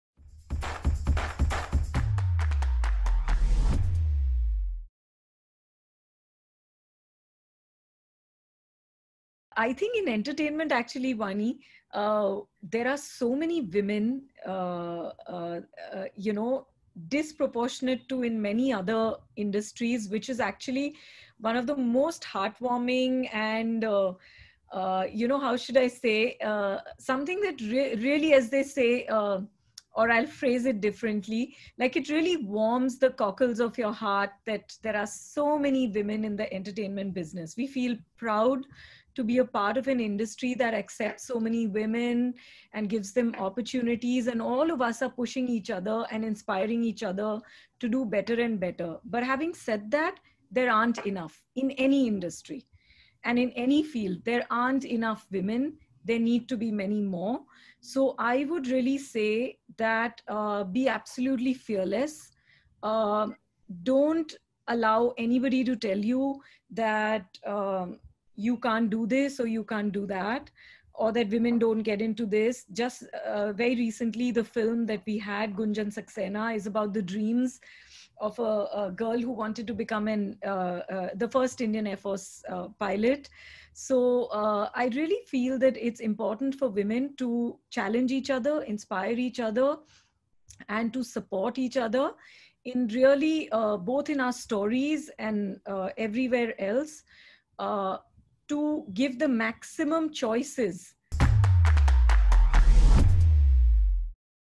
Short Byte | Monika Shergill's advice to women on building careers
Monika Shergill's advice to women on building
1. Be fearless
2. Don't allow anybody to tell you what you can/cannot do
3. Important for women to challenge, support & inspire each other in our stories and otherwise for maximum choices